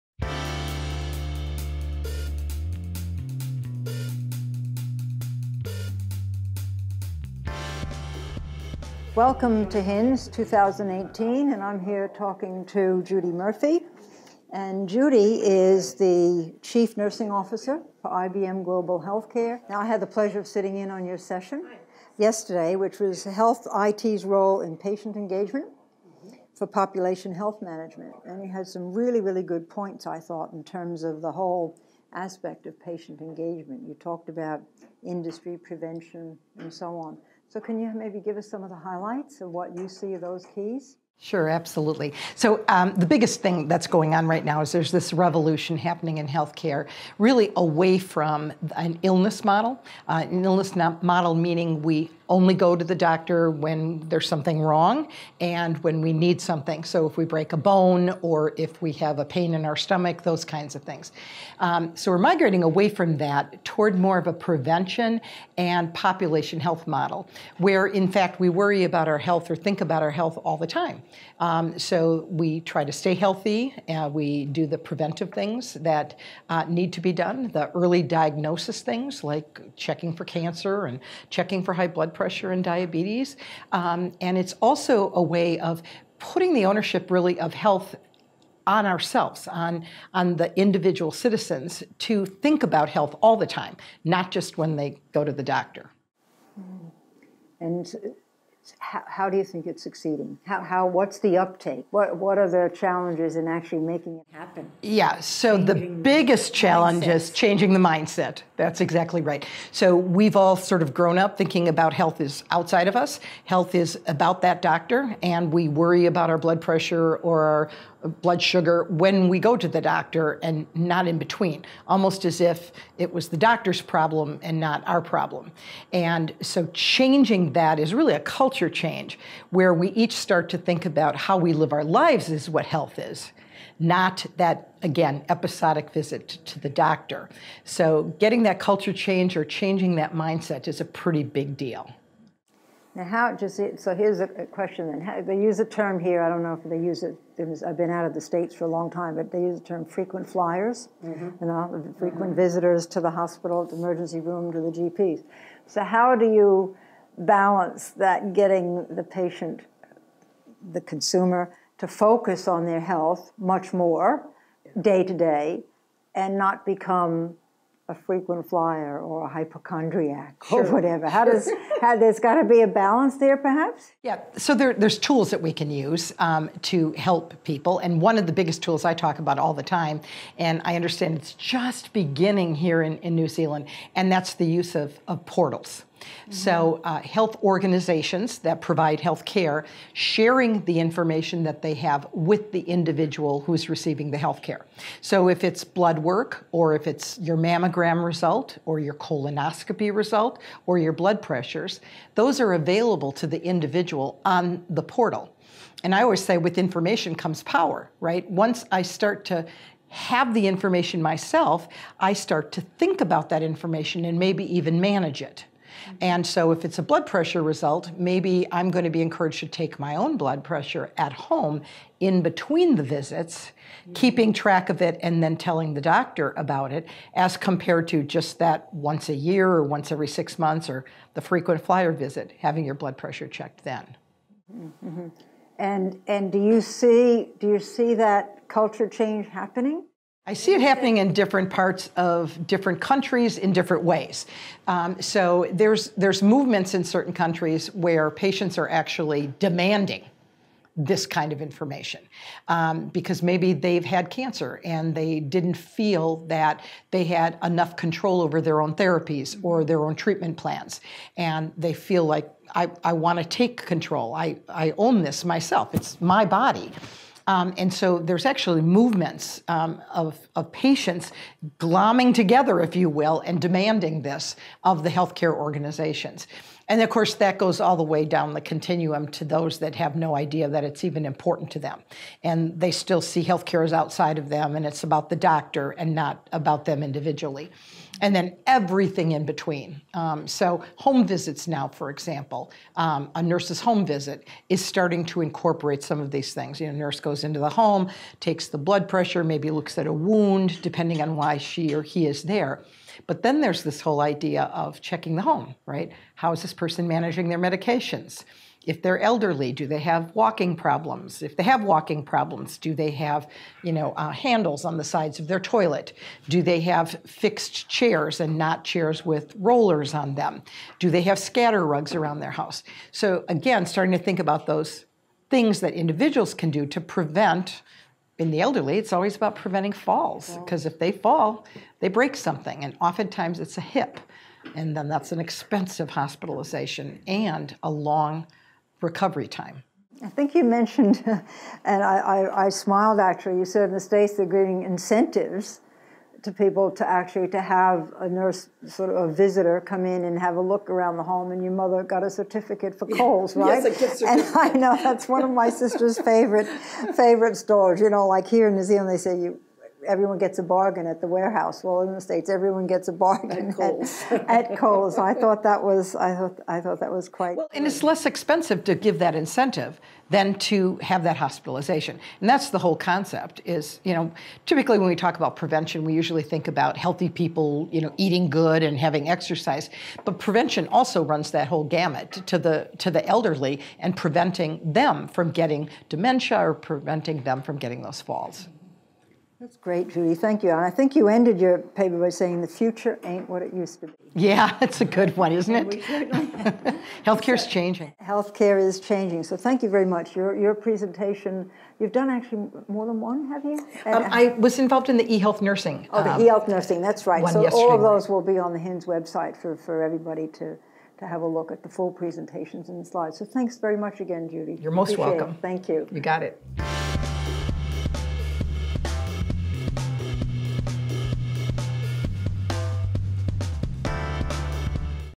HiNZ 2018 Judy Murphy
HiNZ 2018 with Judy Murphy who is the Chief Nursing Officer for IBM Global Healthcare and talks about Health I.T.s role in patient engagement for population health management